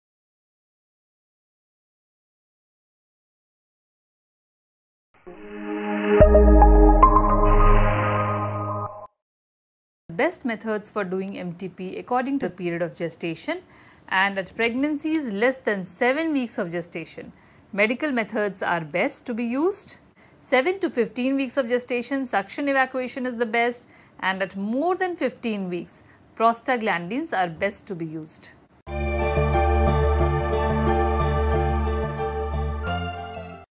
NEETPG-Topic-Best Methods Of MTP -Obs & Gynae
To view more videos download our NEETPG app

DescriptionPeriod of pregnancy Methods
7 weeks Medical method
7-15 weeks Suction evacuation
 15 weeks Prostaglandins